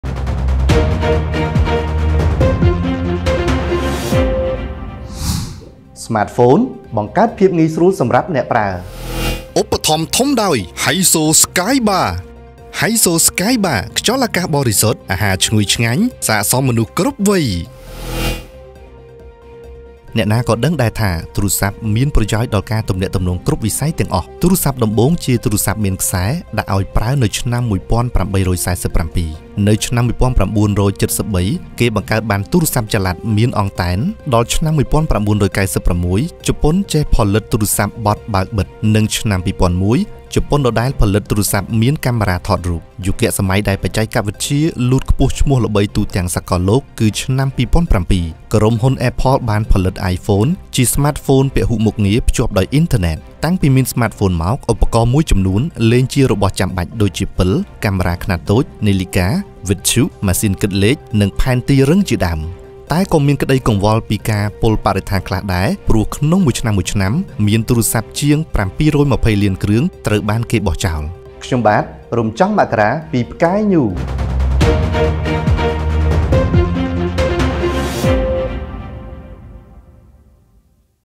ប្រវត្តិខ្លះៗនៃការបង្កើត Smart Phone ដែលអ្នកមិនធ្លាប់បានដឹង!
ប្រវត្តិខ្លះៗនៃការបង្កើត ស្មាតហ្វូន ដែលអ្នកមិនធ្លាប់បានដឹង!
(ស្មាតហ្វូន បង្កើតភាពងាយស្រួល សម្រាប់អ្នកប្រើ) អ្នកណាក៏ដឹងដែរថា ទូរសព្ទមានប្រយោជន៍ដល់ការទំនាក់ទំនង គ្រប់វិស័យទាំងអស់។ ទូរសព្ទដំបូងជាទូរសព្ទមានខ្សែ ដាក់ឱ្យប្រើនៅឆ្នាំ1847 នៅឆ្នាំ1973គេបង្កើតបានទូរសព្ទចល័ត មានអង់តែន ដល់ឆ្នាំ1996 ជប៉ុនចេះផលិតទូរសព្ទបត់ បើក-បិទ និងឆ្នាំ2001 ជប៉ុនដដែល ផលិតទូរសព្ទមានកាំម៉េរ៉ាថតរូប។ យុគសម័យដែលបច្ចេកវិទ្យាលូតខ្ពស់ឈ្មោះល្បីទូទាំងសកលលោកគឺឆ្នាំ2007 ក្រុមហ៊ុនappleបានផលិតIphone ជាស្មាតហ្វូនពហុមុខងារ ភ្ជាប់ដោយinternet ។ តាំងពីមានស្មាតហ្វូនមក ឧបករណ៍១ចំនួន លែងជារបស់ចាំបាច់ ដូចជា ពិល កាំមេរ៉ាខ្នាតតូច នាឡិកា វិទ្យុ ម៉ាស៊ីនគិតលេខ និងផែនទីរឹងជាដើម។ តែក៏មានក្តីកង្វល់ពីការពុលបរិស្ថានខ្លះដែរ ព្រោះ_ក្នុង១ឆ្នាំៗ មានទូរសព្ទជាង720លានគ្រឿង ត្រូវបានគេបោះចោល។